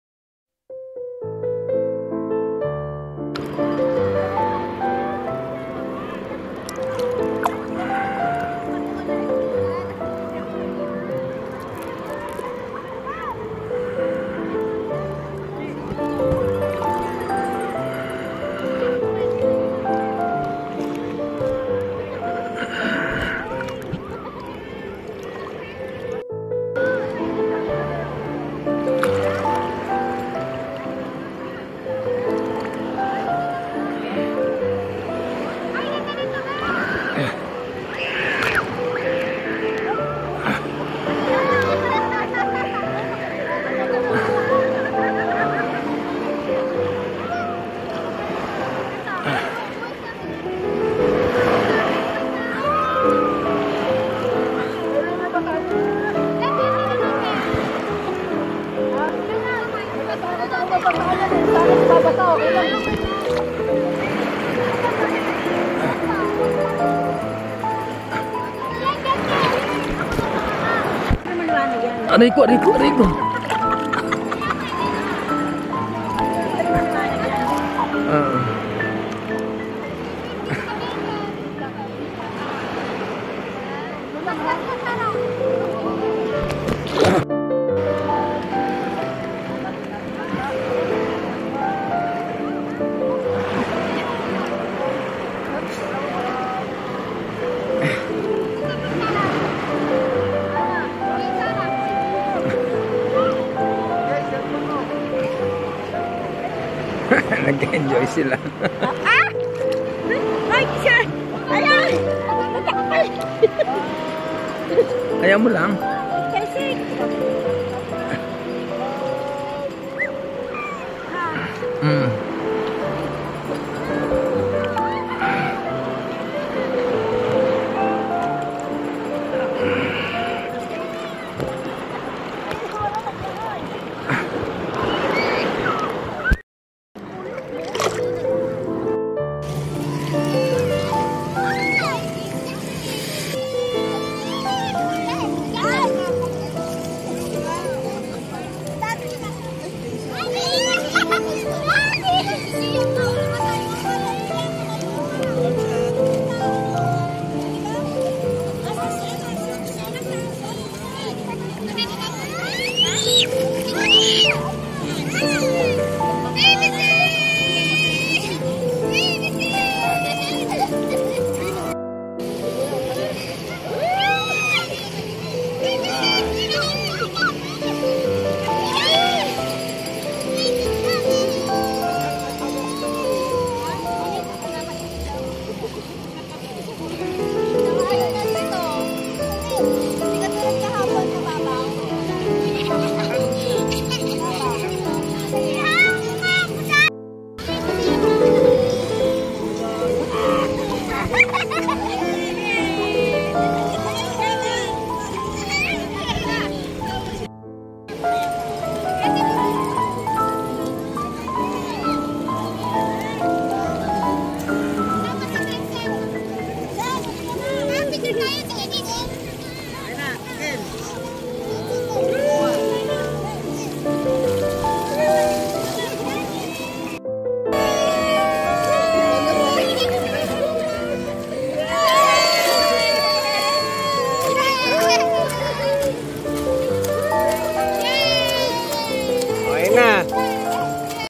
GOOD TIMES AND MEMORIES IN THE BEACH | LAIYA BEACH RESORT | SAN JOSE BATANGAS
Family outing at Laiya Beach Resort, San Jose Batangas